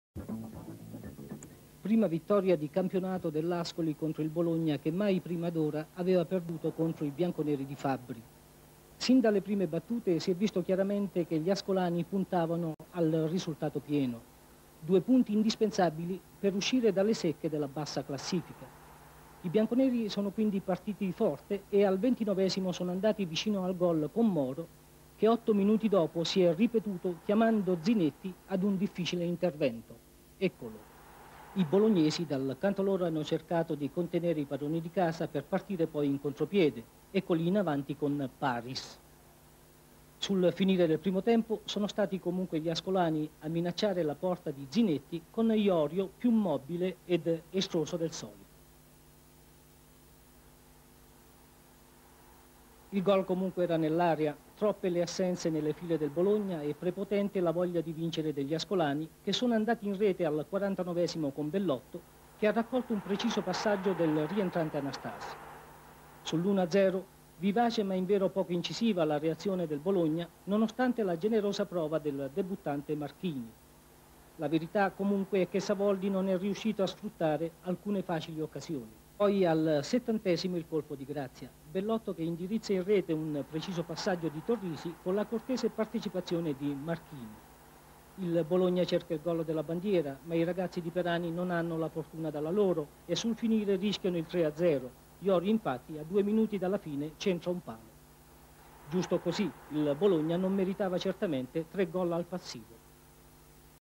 Ascoli-Bologna 2-0 Serie A 79-80 7' Giornata
49'  BELLOTTO
70'  AUTOGOL MARCHINI